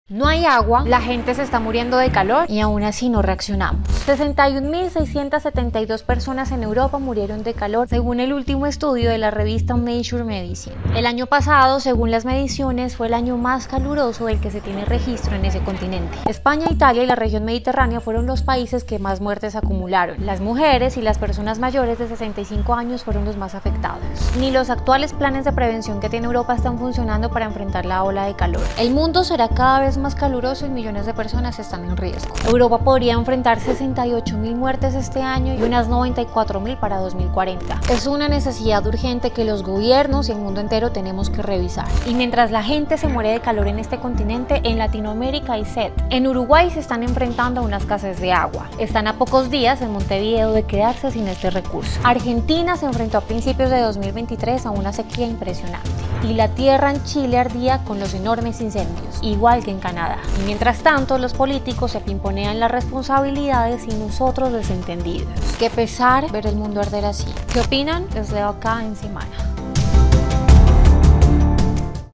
Cerca de 62.000 personas en Europa murieron de calor en 2022, según un estudio
Según la revista Nature Medicine, cerca de 62.000 personas en algunos países de Europa murieron en la oleada de calor en 2022. Millones están en riesgo. Paola Vega tiene los detalles.